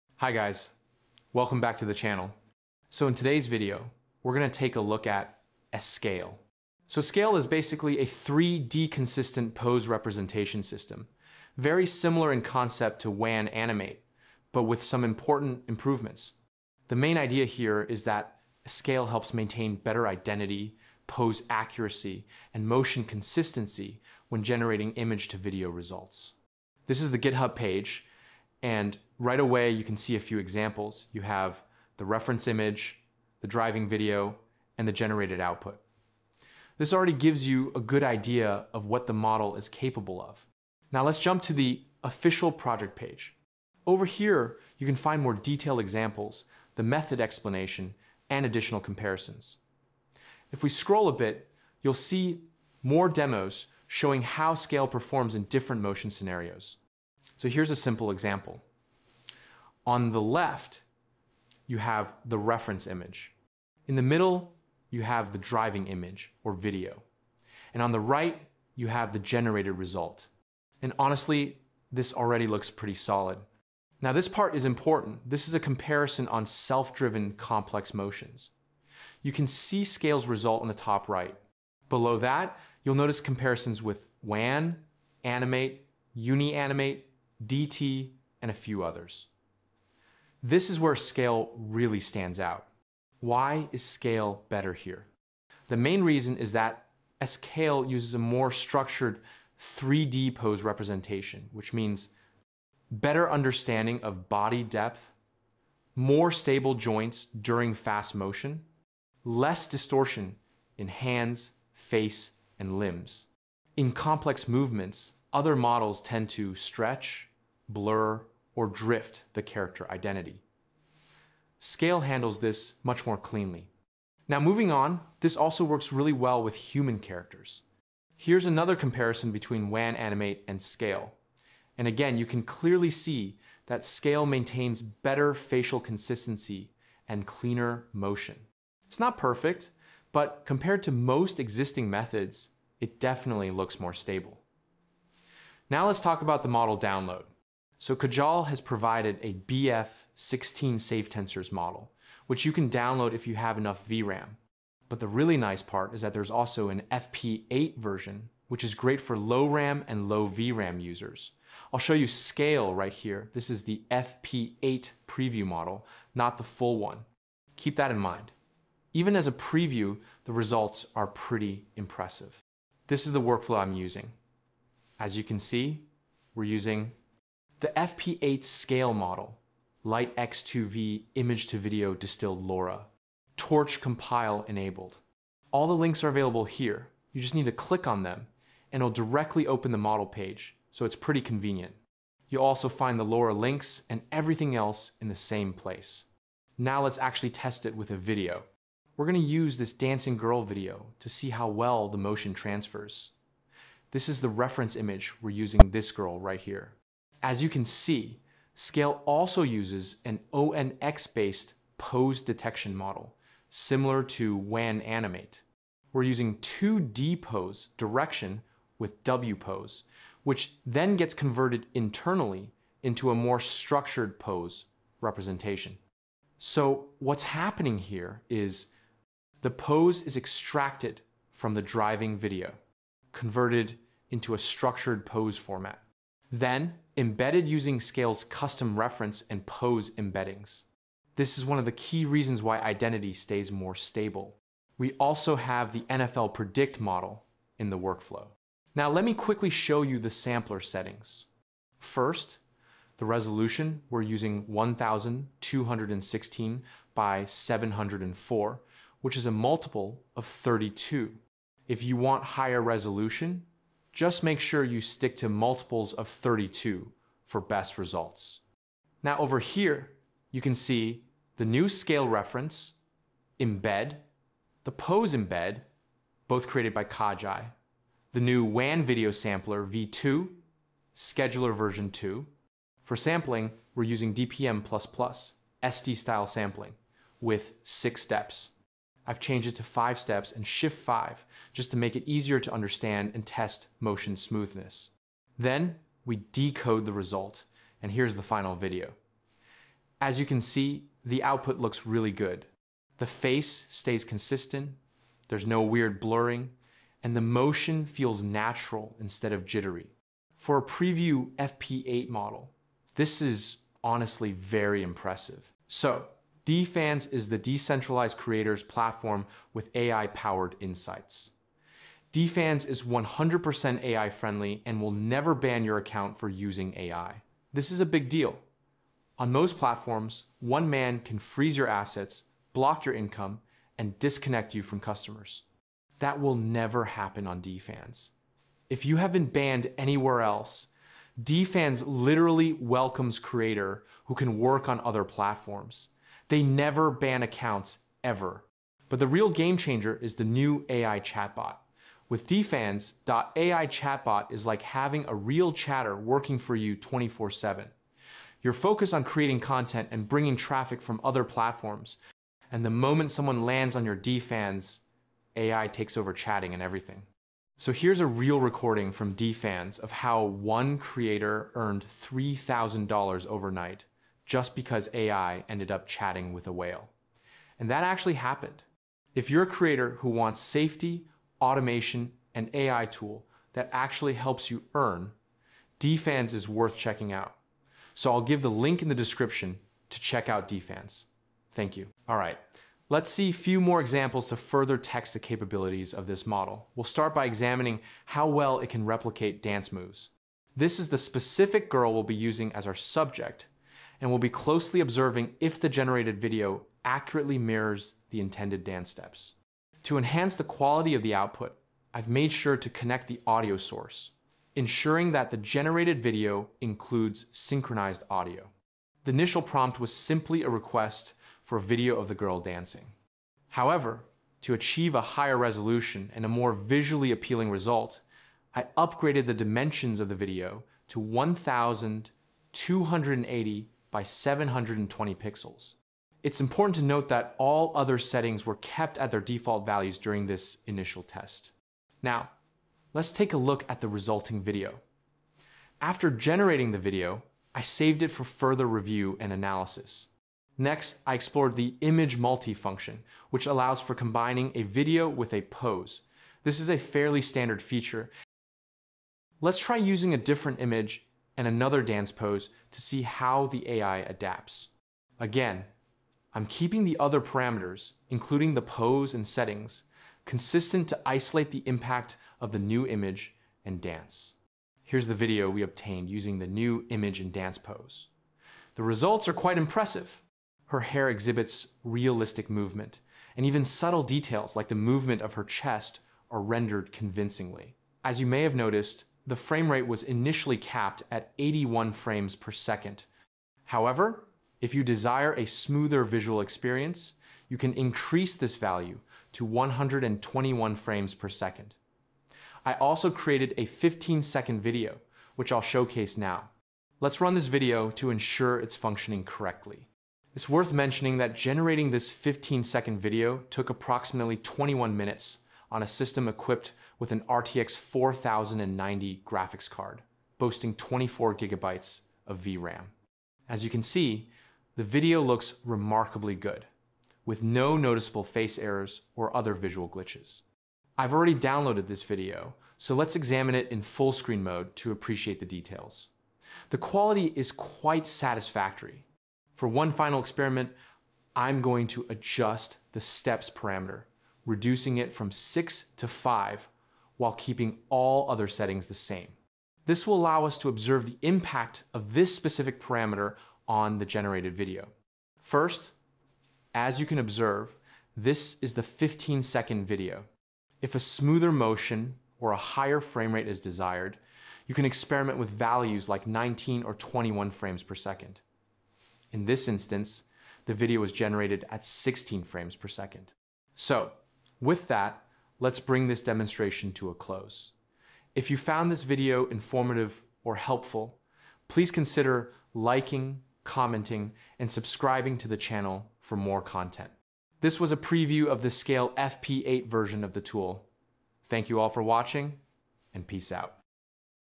SCAIL in ComfyUI – Next-Gen 3D Motion Control for AI Animation
In this video, we explore how to use SCAIL inside ComfyUI to achieve next-gen 3D motion and pose control for character animation.

I’ll walk through:

setting up SCAIL in ComfyUI

how 3D pose inputs work

generating consistent full-body character motion

studio-grade animation workflows

tips to avoid motion artifacts + jitter

SCAIL brings studio-level animation into an open-source workflow, letting you animate characters without traditional rigging or timeline keyframes. If you're building AI animation pipelines, using pose controllers, or pushing realism in motion — this tutorial will help you level up fast.

👇 comment if you want the workflow file